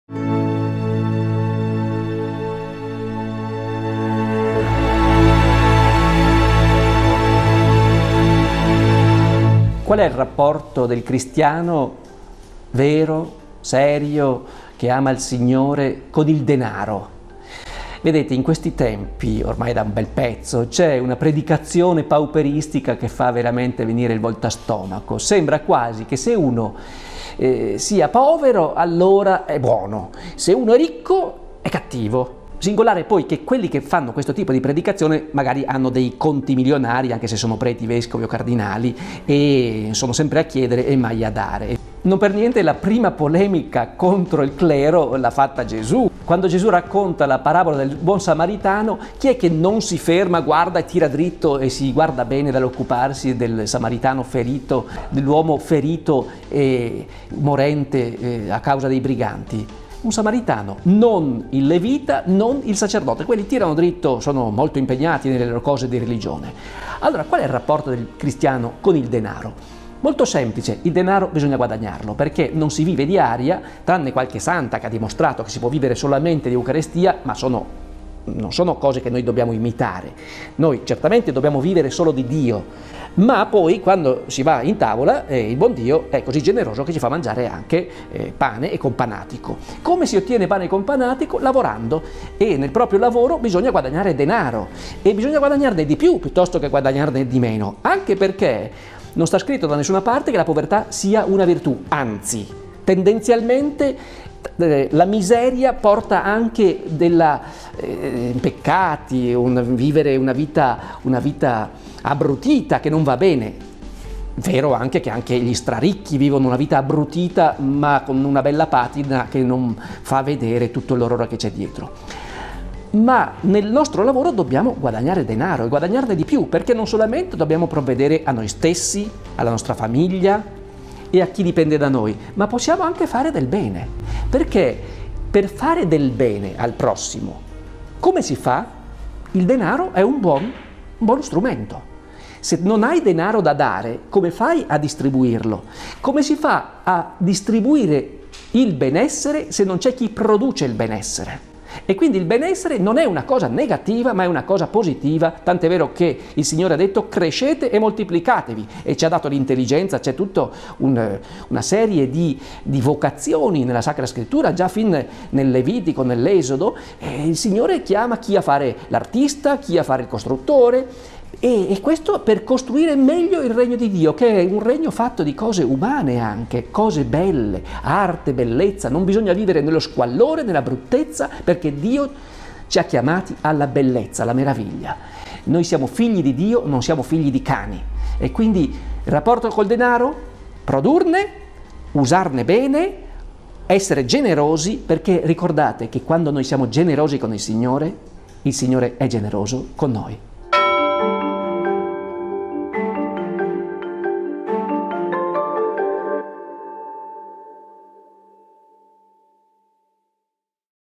Che rapporto avere col denaro?
Sei proprio sicuro che guadagnare di più sia sinonimo di essere un cattivo cristiano? Questo non è mai stato l'insegnamento della Chiesa. Scopriamo insieme quale rapporto avere coi beni terreni.
Leggi "Amare Dio e fare soldi" di Ettore Gotti Tedeschi